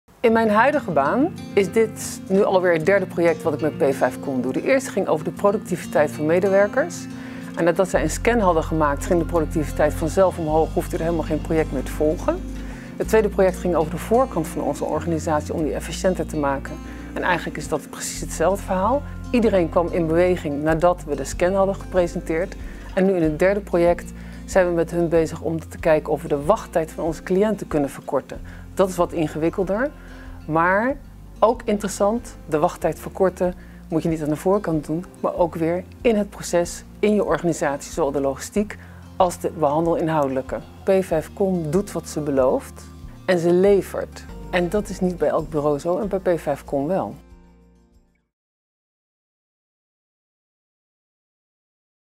Interview met Hanneke Haanraadts en Anette Pet | Directie Helen Dowling Instituut |Testimonial
Directeur Hanneke Haanraadts en Adjunct-Directeur Anette Pet van het Helen Dowling Instituut over hun ervaringen en relatie met P5COM. De voldoening van meer dan opdrachtgever en opdrachtnemer zijn.